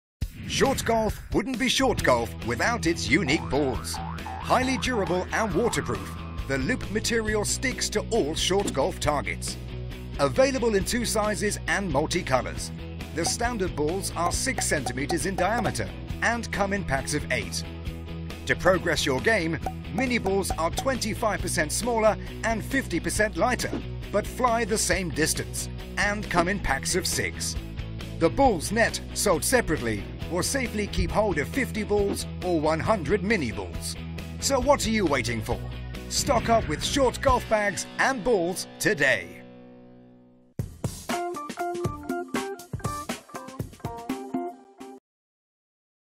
u4golf SHB Short Golf Ballz + Bag
Large and small low compression balls with loop material covers that stick to ShortGolf targets
Net for storing 50 ballz! or 100 mini-ballz!
Durable, waterproof loop material covers stick to ShortGolf targets
Ballz! are 6cm diameter and supplied in pack of 8 colours
Ballz! weigh same as regulation golf ball to give correct feel with hitta! and putta!
Mini-ballz! provide skill progression from ballz!
Supplied in pack of 6 colours
Mini-ballz! 25% samller and 50% lighter than ballz!, but fly same distance
Ballz! Net is colourful storage bag for 50 ballz! or 100 mini-ballz! (ballz! not included).